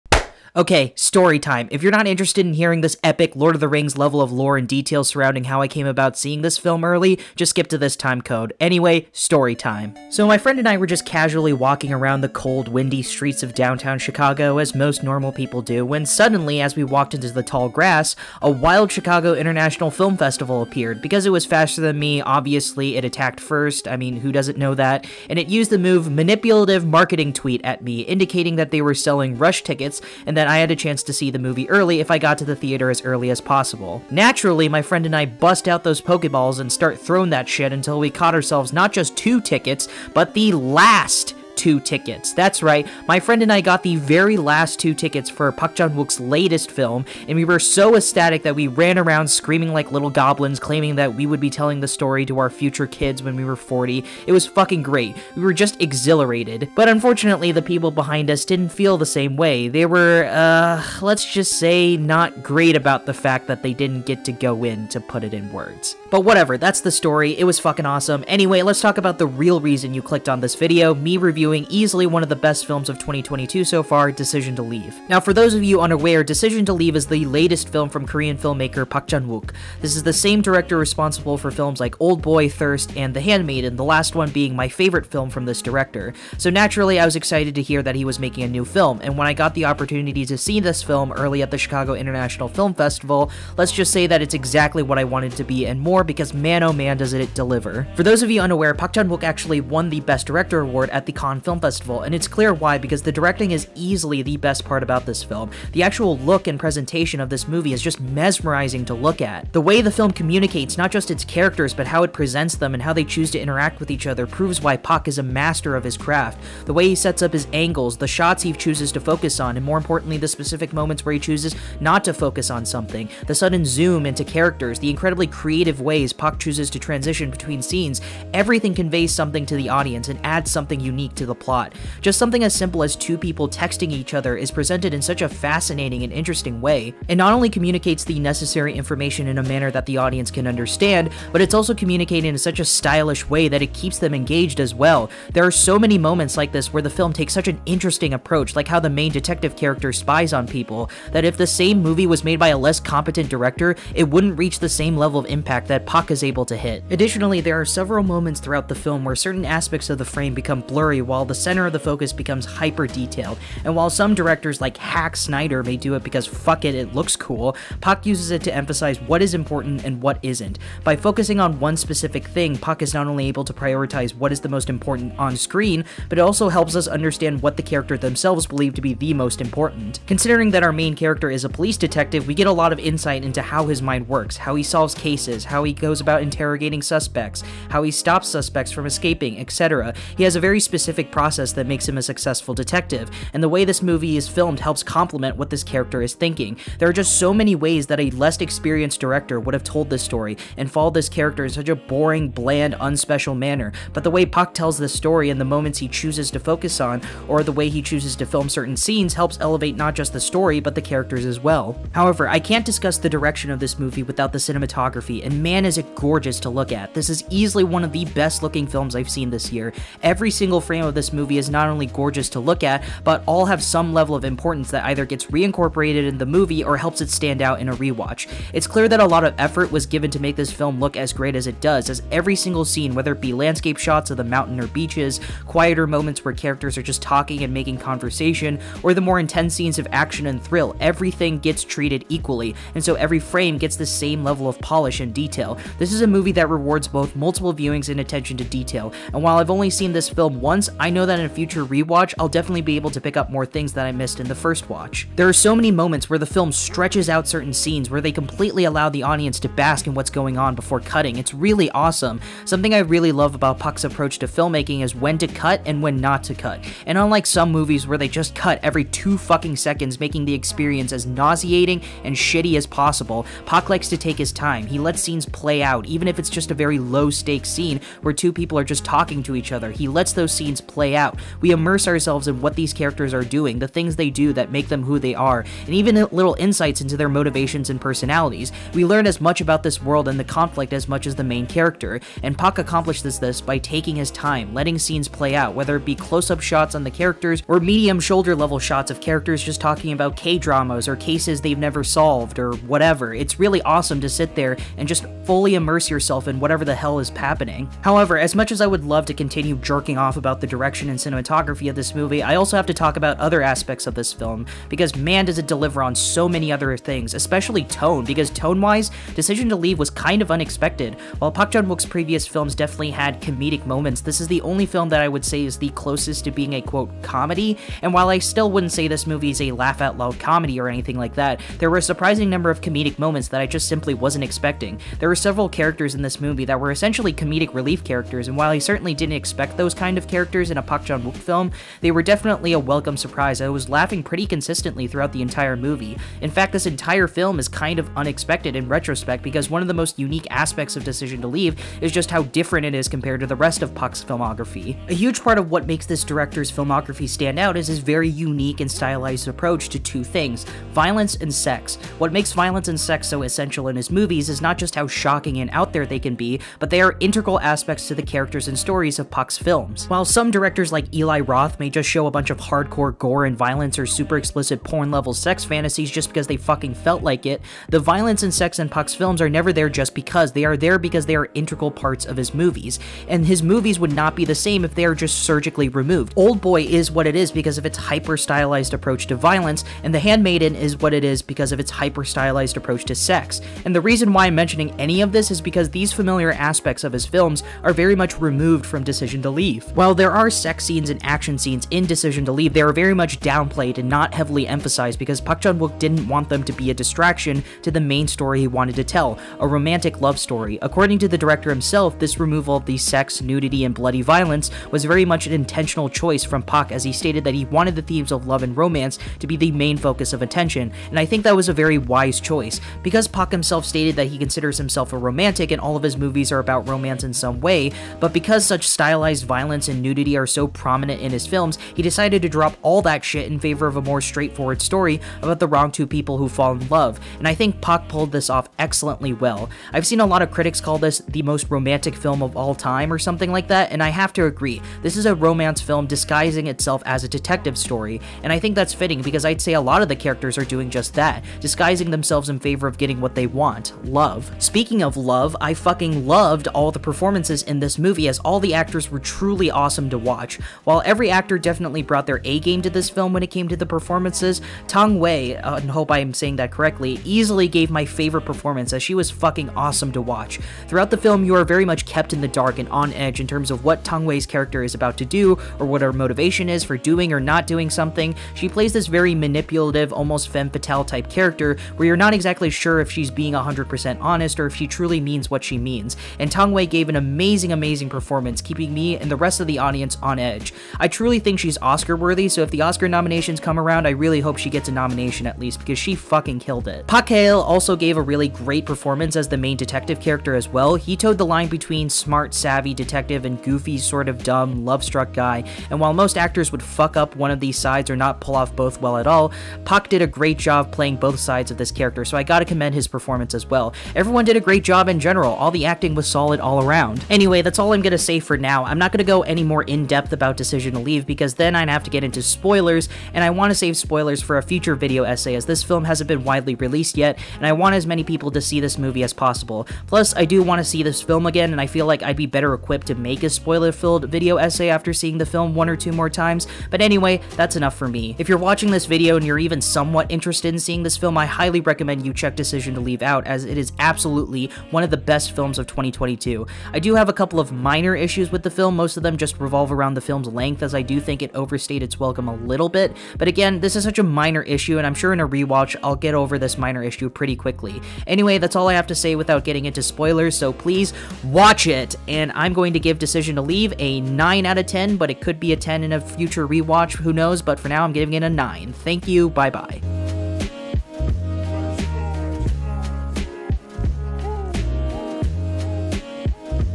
Why You Should Watch Park Chan-wook's Decision to Leave- Chicago Int. Film Festival (No Spoilers)
Definitely the best film of 2022 made by a guy named Park Chan-wook.

0:00- Introduction
1:14- Review